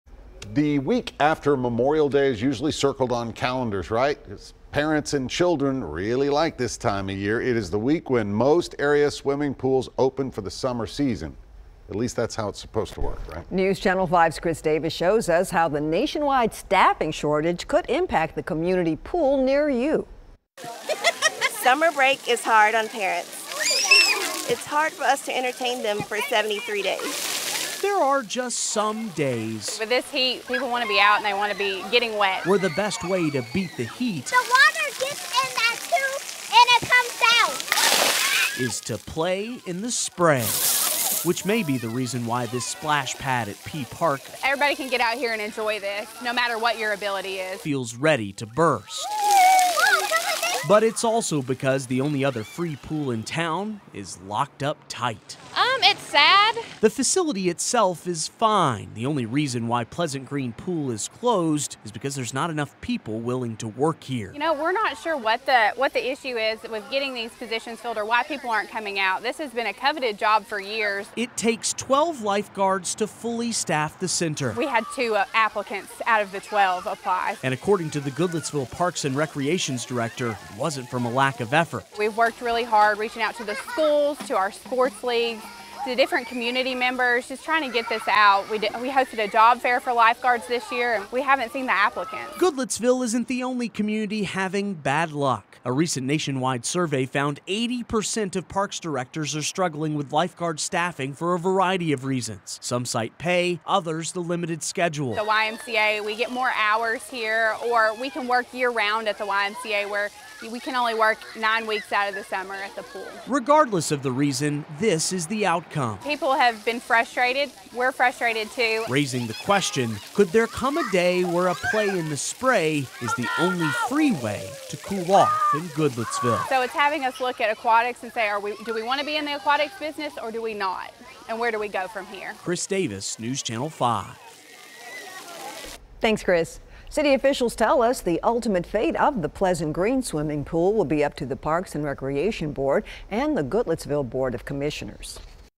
Goodlettsville keeping community pool closed this summer due to lifeguard shortage
There are just some days when the best way to beat the heat is to play in the spray.